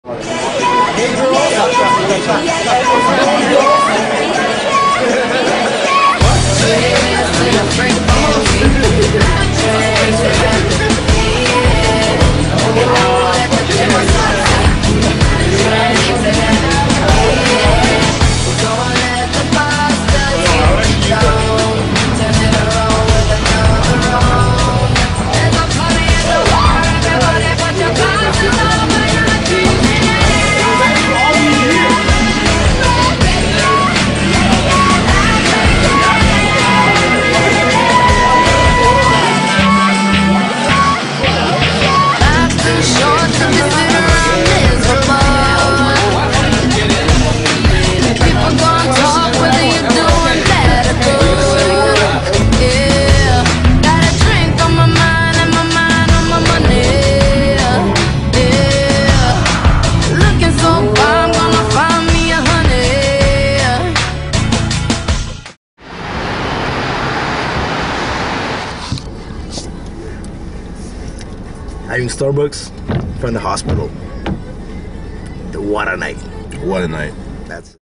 Halloween Party | Toronto | 2012
Prior to going clubbing for Halloween, we all meet up at a downtown Toronto hotel to pre-drink. Enjoy.